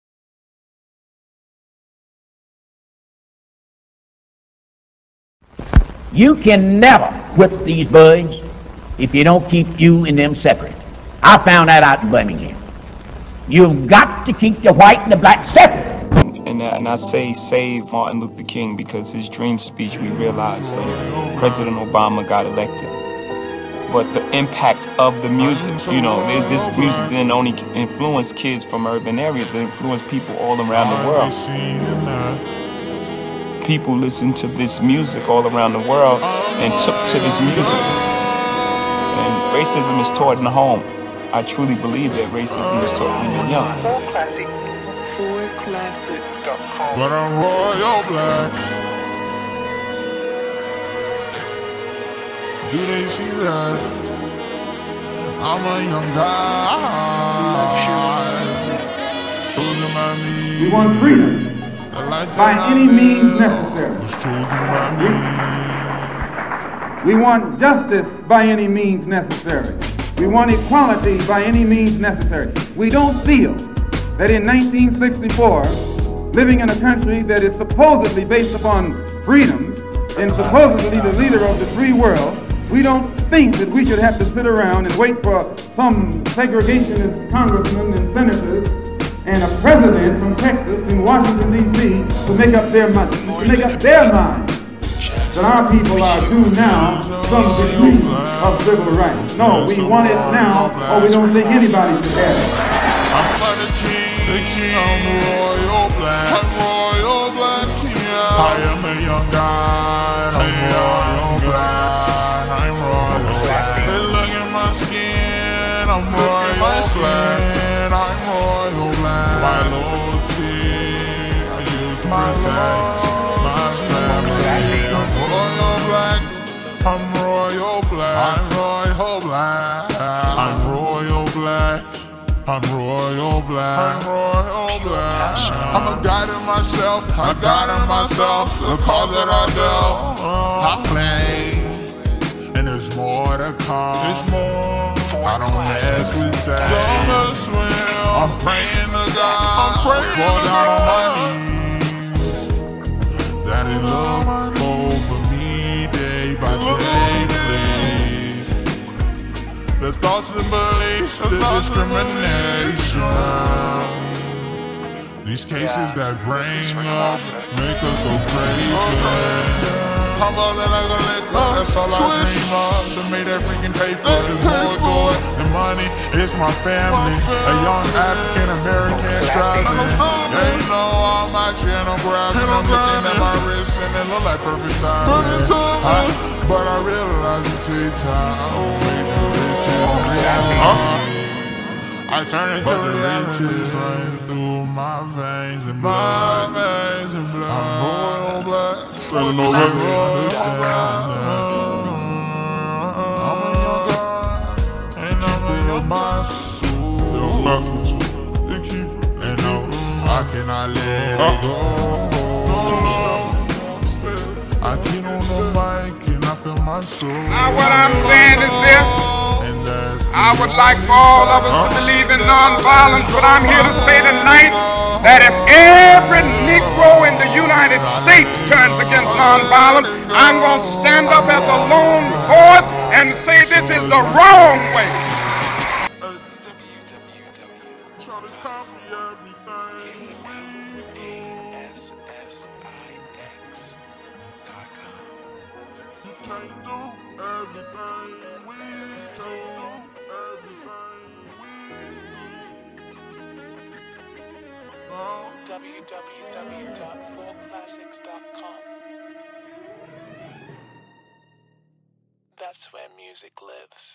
Royal Black Black History
Inspired by African American trials and tribulations. Yet, We found solace in togetherness. Thru Spiritual Belief, Music, Sports, Building, Etc. You name it we broke the barrier. So please enjoy the educational visual!
Thanks for your Support, FMEAgent Marco Polo
Visual By: Marco Polo Montana